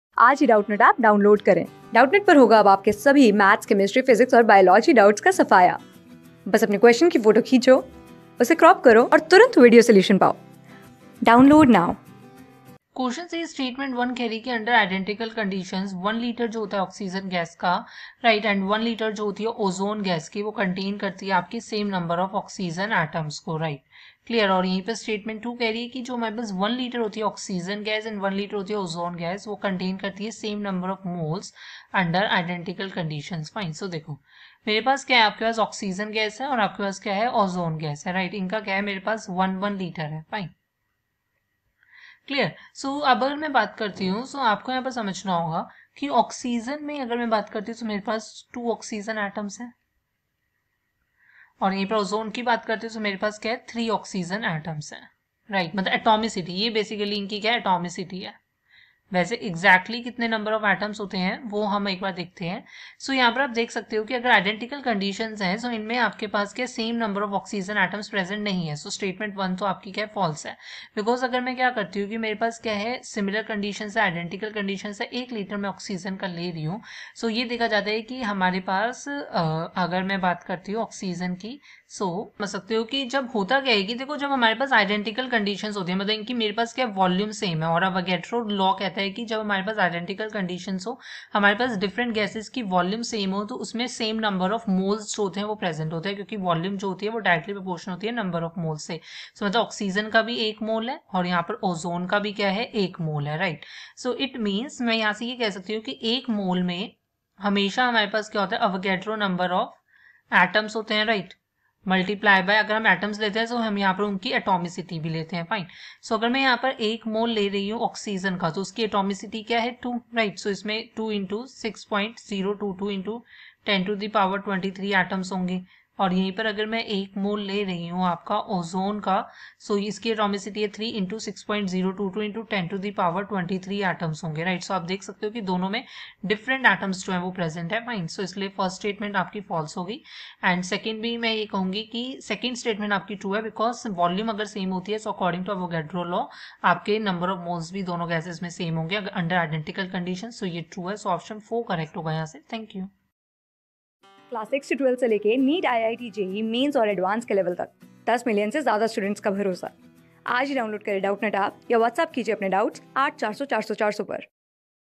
Statement-1. Under identical conditions, 1 L of O_(2) gas an 1 L of O_(3) gas contain the same n...
Statement-1. Under identical conditions, 1 L of O_(2) gas an 1 L of O_(3) gas contain the same number of oxygen atoms. Statement-2. 1 L of O_(2) and 1 L of O_(3) contain the same number of moles under identical conditions.
Class: 12
Subject: CHEMISTRY
Chapter: SOME BASIC CONCEPTS OF CHEMISTRY
Board:CBSE

👉 Have Any Query? Ask Us.